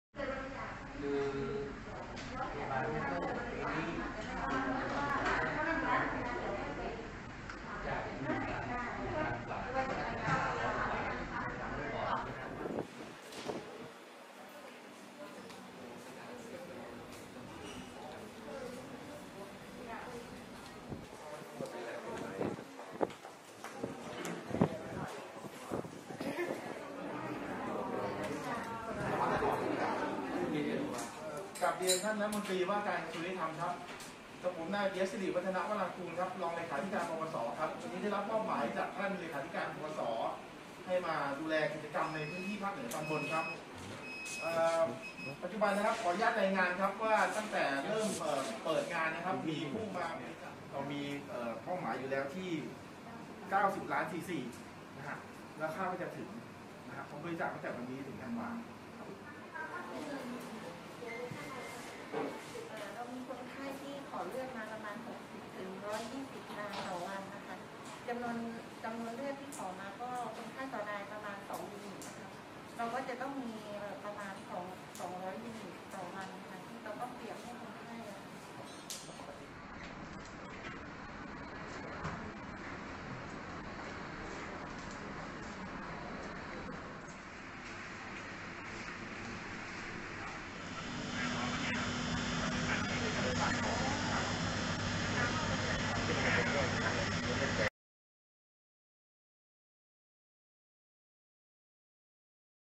กิจกรรมบริจาคโลหิต “90 พรรษา 90 ล้านซีซี
ปปส.ภาค 5 จัดกิจกรรมบริจาคโลหิต “90 พรรษา 90 ล้านซีซี ถวายราชสดุดี สมเด็จพระบรมราชชนนีพันปีหลวง